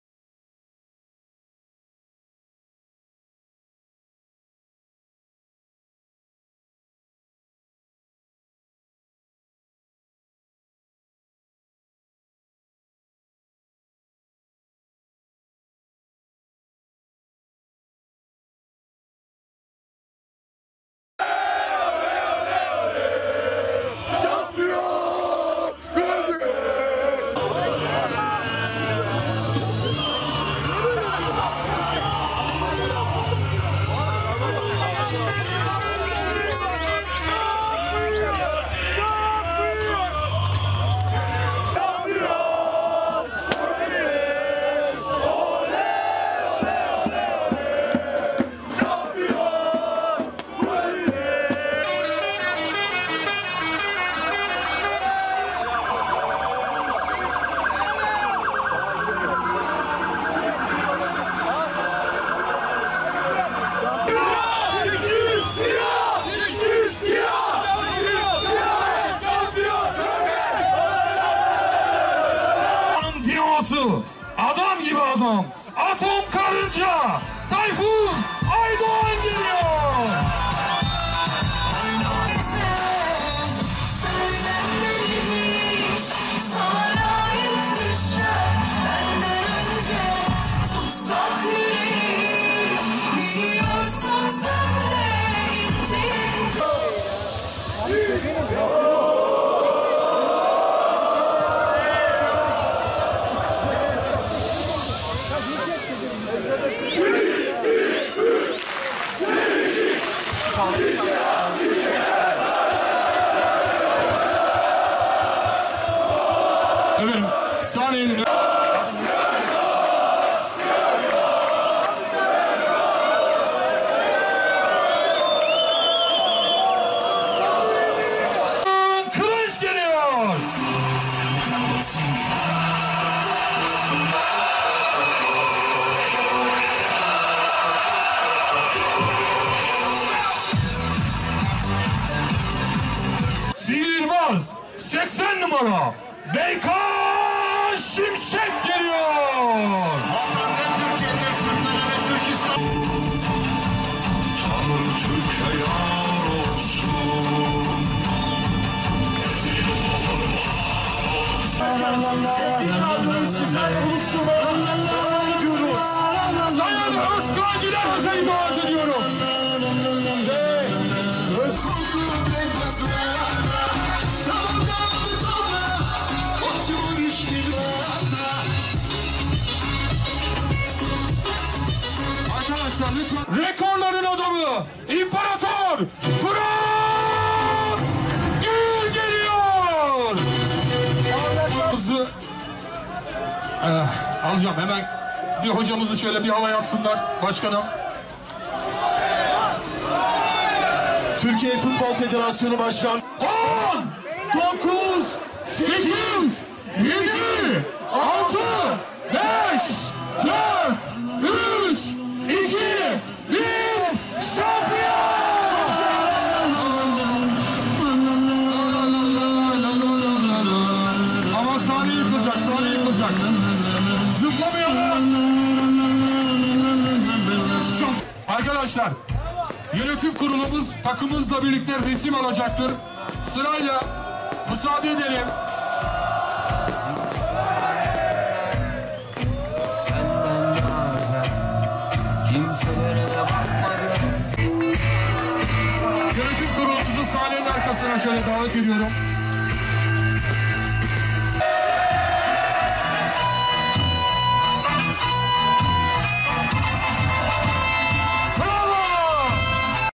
Kocaelispor'dan görülmemiş şampiyonluk kutlaması
TFF 2.Lig’de sezonun bitimine bir hafta kala şampiyonluğunu ilan eden ve geçen yıl düştüğü TFF 1.Lig’e yeniden yükselen Kocaelispor binlerce taraftarıyla birlikte kupa kaldırdı. Kupa gecesinde Kocaelispor’un uzun vadeli başarısı için gerçekleştirilen yapılanmaya dair sinyaller verildi.